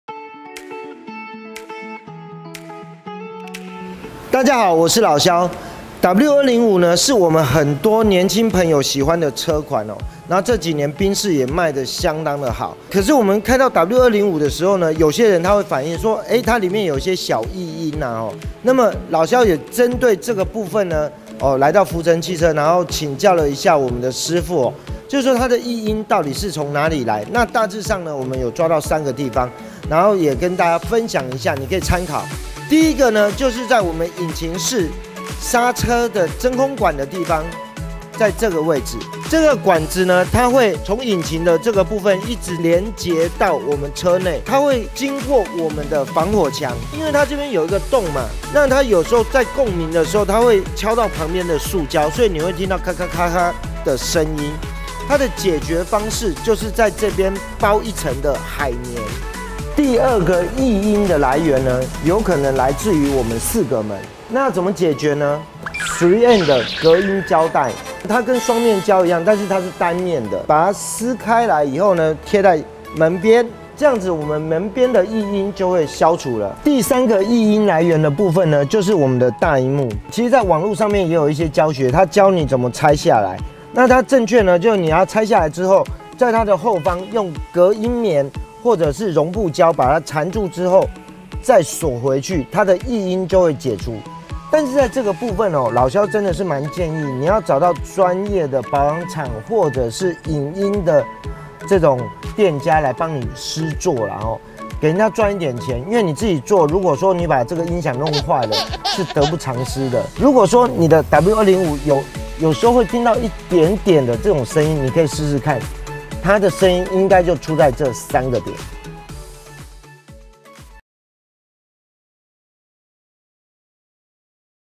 Benz W205 C300哪3個地方容易出現異音？要怎麼解決異音問題？【老蕭汽車小常識】
各位W205 C300車主的心聲，老蕭都聽到了！
這次老蕭帶大家到福誠，不僅帶你看到異音從哪裡來，還手把手教學帶你解！
看完影片後有異音問題的朋友趕快去試試吧！